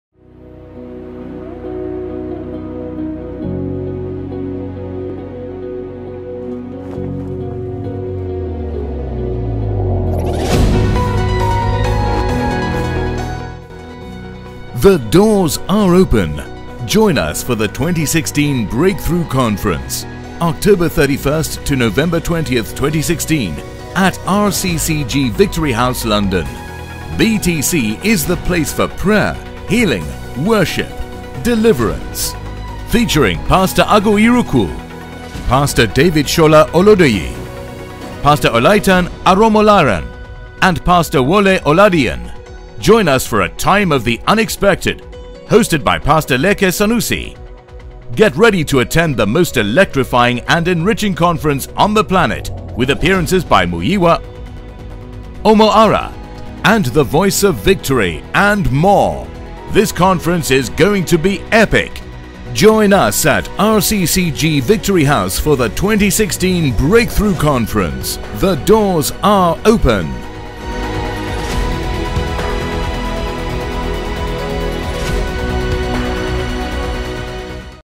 THE DOORS ARE OPEN BTC2016 2
THE DOORS ARE OPEN

JOIN US FOR THE 2016 BREAK THROUGH CONFERENCE

OCT 31ST - NOV 20TH, 2016
AT RCCG VICTORY HOUSE LONDON

BTC IS THE PLACE FOR PRAYER, HEALING, WORSHIP, DELIVERANCE…

FEATURING
PASTOR AGU IRUKWU
PASTOR DAVID SOLA OLUDOYI
PASTOR OLAITAN AROMOLARAN
& PASTOR WOLE OLADIYUN

JOIN US FOR A TIME OF THE UNEXPECTED
HOSTED BY PASTOR LEKE SANUSI

GET READY TO ATTEND THE MOST ELECTRIFYING AND ENRICHING CONFERENCE ON THE PLANET
WITH APPEARANCES BY MUYIWA OLAREWAJU, OMO ARA, SEYI ALESH, THE BEKES AND THE VOICE OF VICTORY AND MORE…

THIS CONFERENCE IS GOING TO BE EPIC!

JOIN US AT RCCG VICTORY HOUSE FOR THE 2016 BREAK THROUGH CONFERENCE.